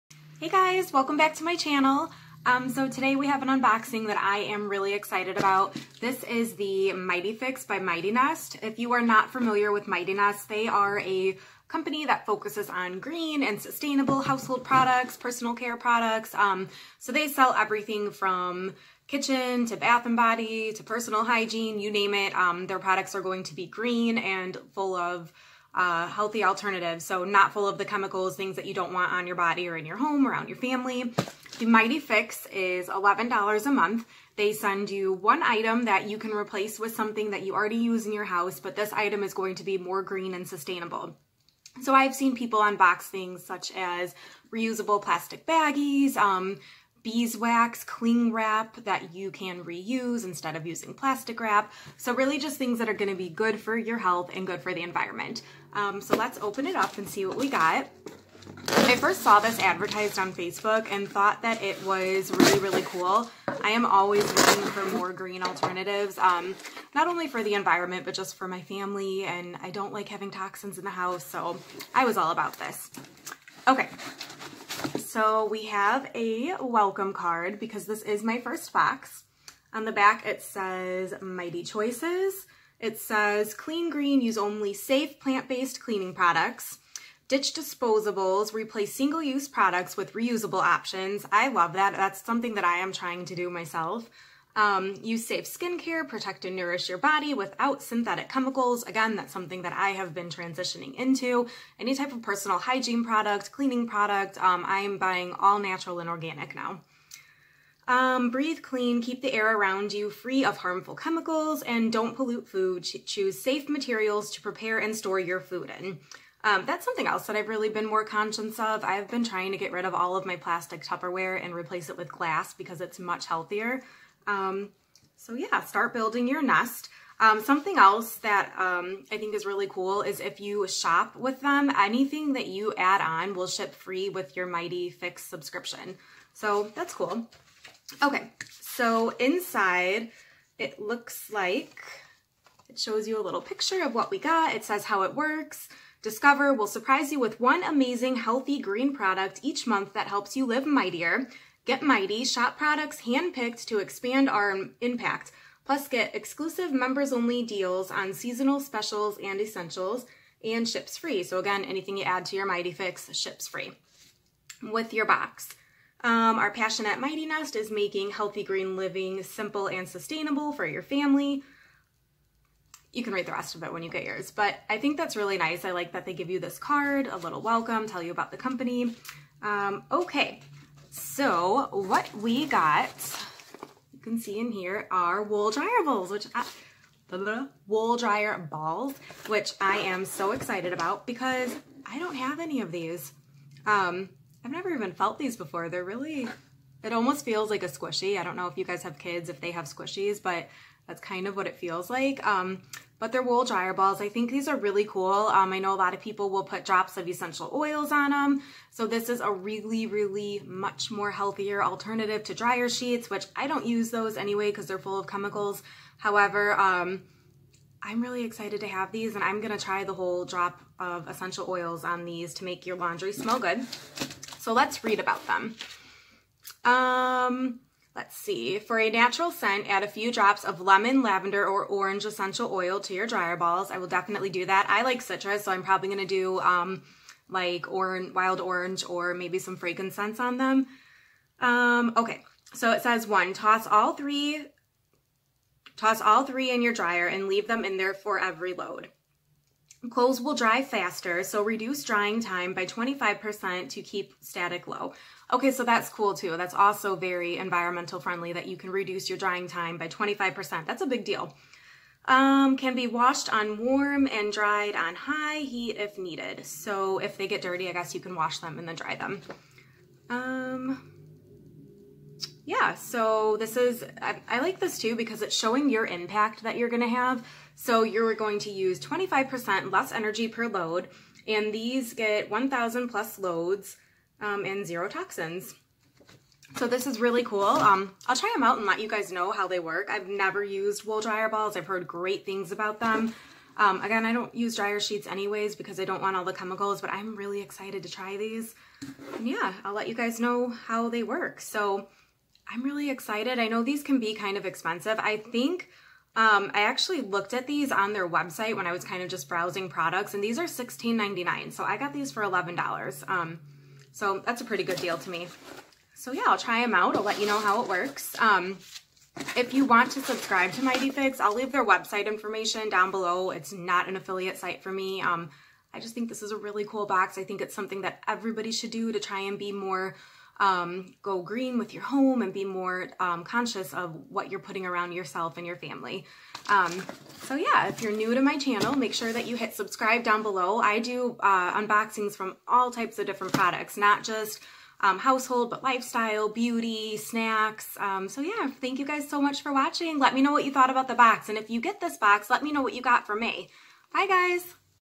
Mighty Fix unboxing May 2019 🌿
Today we are unboxing the Mighty Fix by Mighty nest. Mighty Fix is a monthly subscription service for $11/month and they send you one green product for your home!🌿

🏃‍♀️FabFitFun
Join here:
$10 off your first box

💄LIV Beauty Box
Join here: